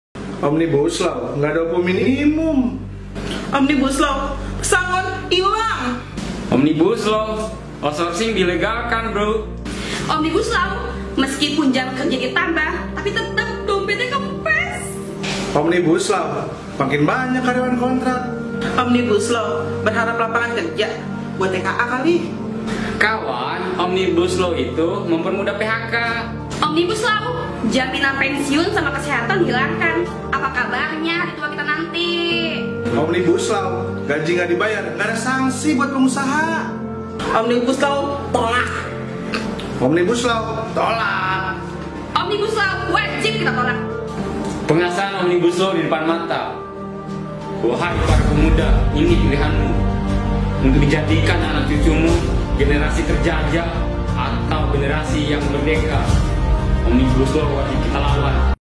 yuk viralkan ini lah provokator penyebar hoak omnibuslow.
Yuk kita viralkan. Mukanya jelas, polisi mestinya mudah nangkepin orang-orang ini. Provokator penyebar hoax.